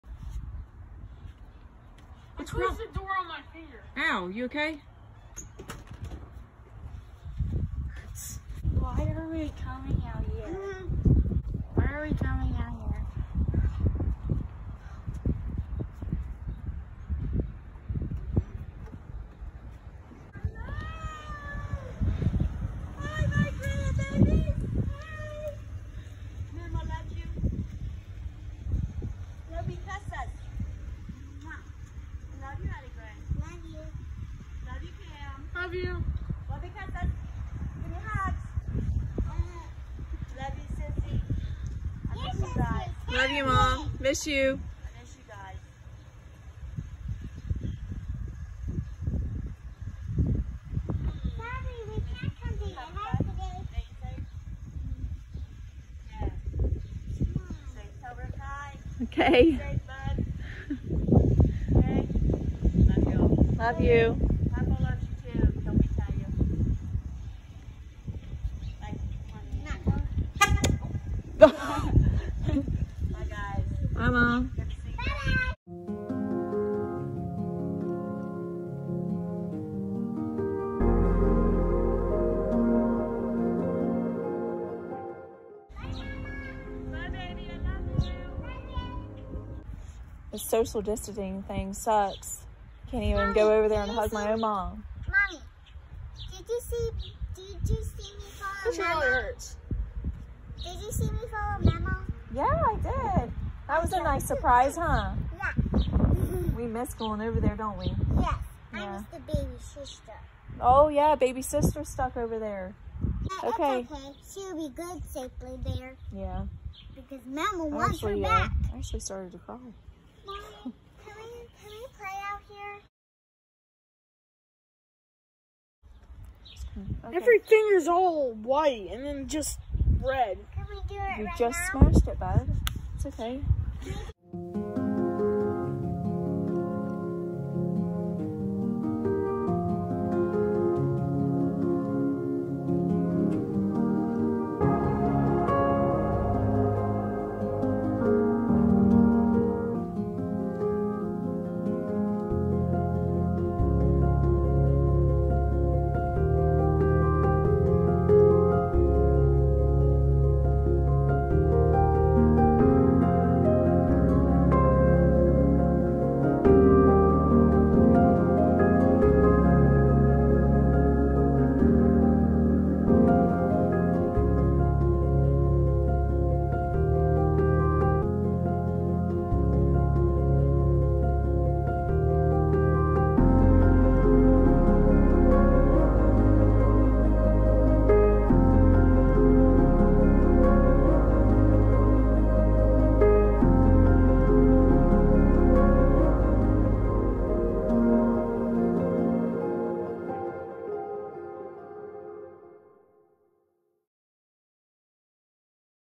For My Parents
Featuring: kylah, Adilynn, Kam, me, and my mom

With the pandemic at its peek we have to continue social distancing. It is very difficult to do. Can’t hug or visit my family or friends. The world can’t wait til this over!